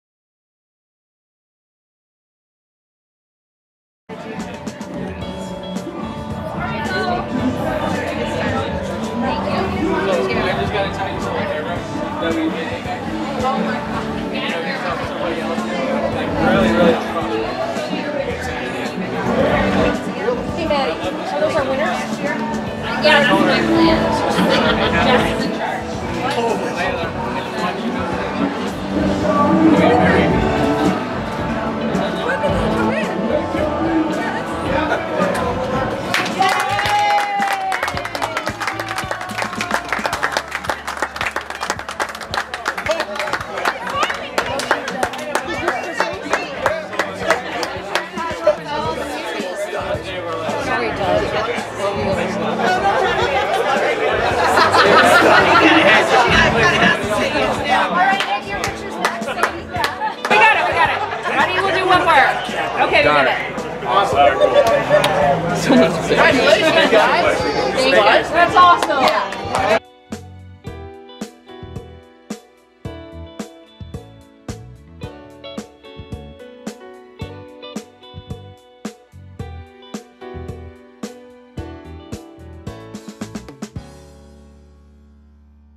Proposal at Justin Moore Meet and Greet Caught on Camera
This guy had a plan to make his proposal unforgettable. Thanks to Justin Moore for making this moment totally unique for two lovebirds!